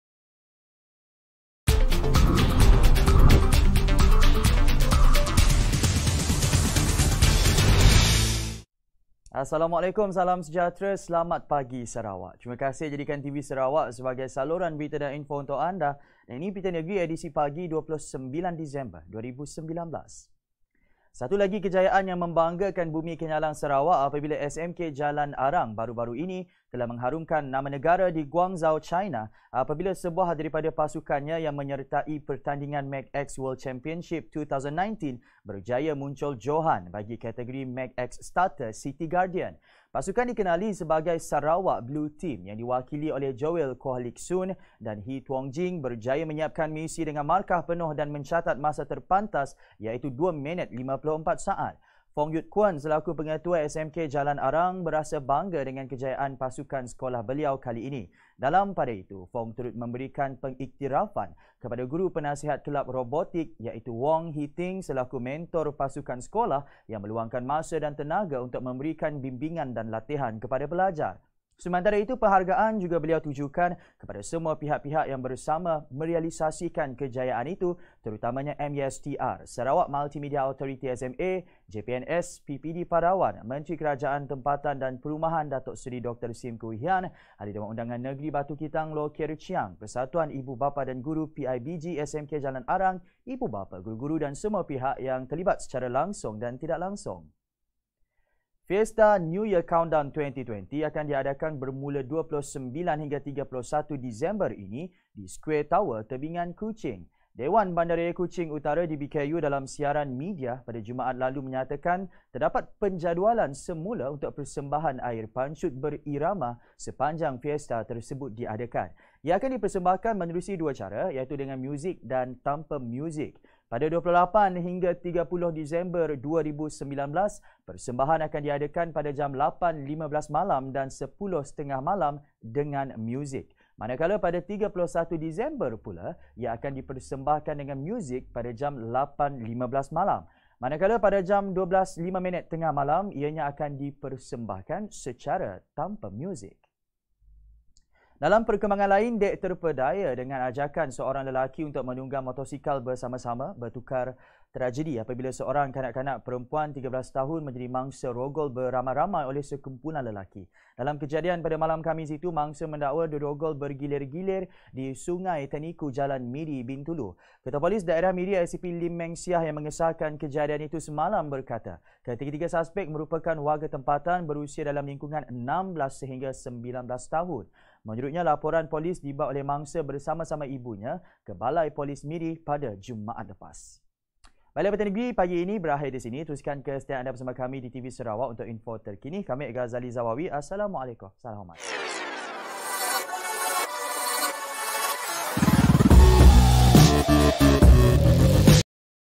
SMK Jalan Arang Harumkan Nama Negara Di China
1. SMK Jalan Arang Harumkan Nama Negara Di China

2. Penjadualan Semula Air Pancut Berirama Di Tebingan Kuching

3. Kanak-Kanak 13 Tahun Dakwa Jadi Mangsa Rogol Berkumpulan